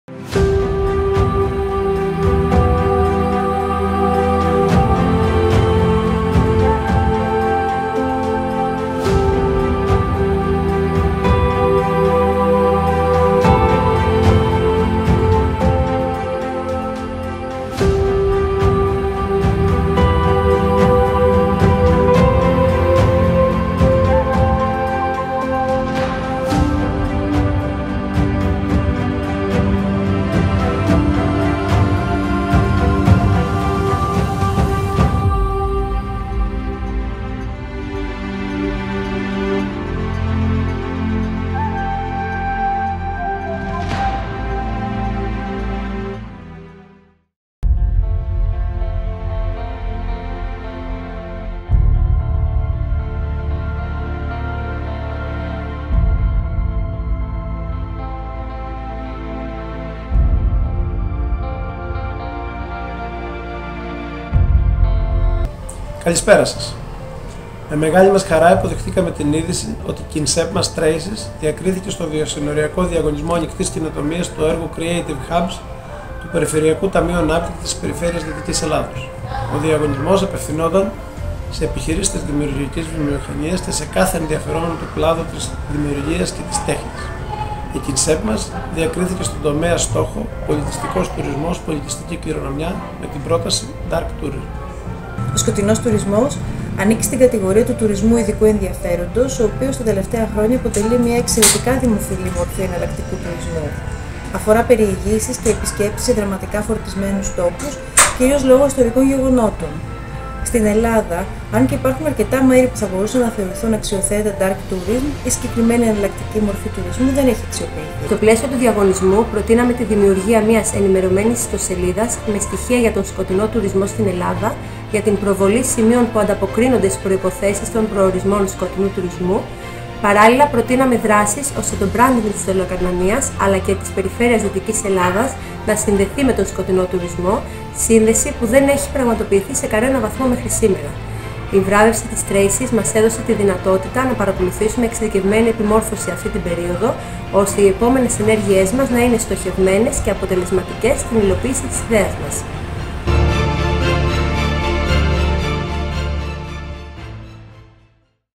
TRACES KOIN.S.EP. - "Dark Τourism in Greece with Aitoloakarnania as a pioneer"
3rd AWARD under "Cultural Tourism/Heritage" Sector, of the Cross-Border Open Innovation Contest!
"Creative@Hubs" - Holistic networking of creative industries via hubs
An Interreg V-A Greece-Italy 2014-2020 Project.